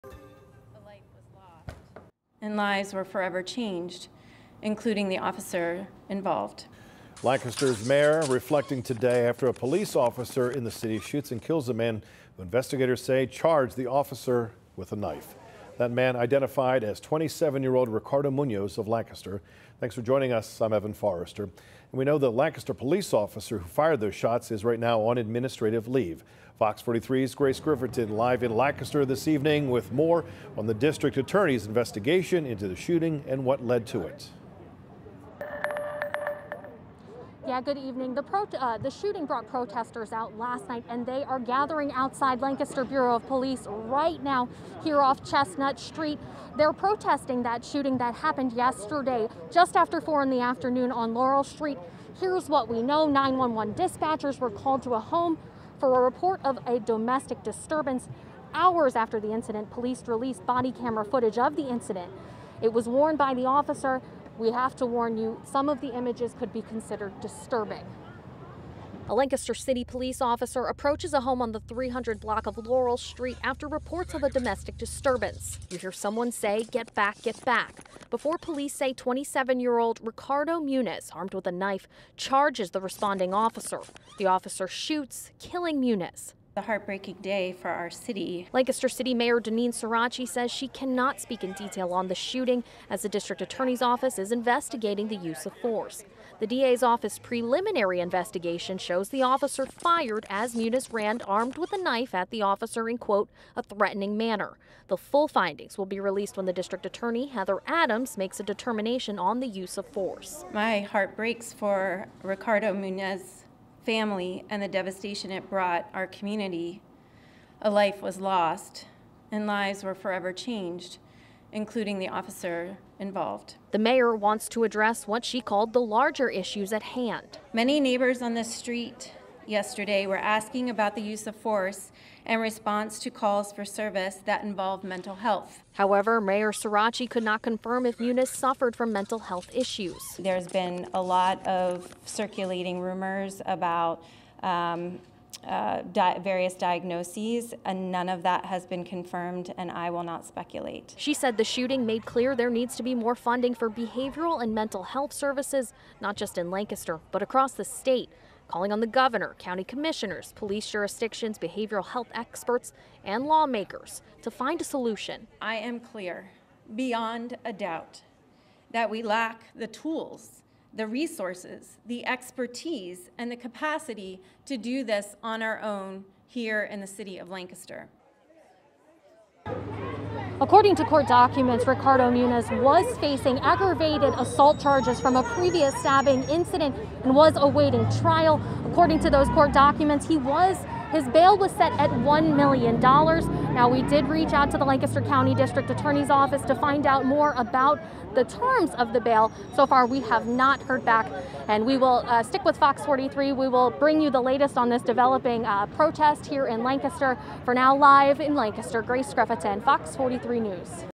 Authorities investigate after 27-year-old man dead after officer-involved shooting in Lancaster
Authorities are investigating after a 27-year-old man was killed in a police-involved shooting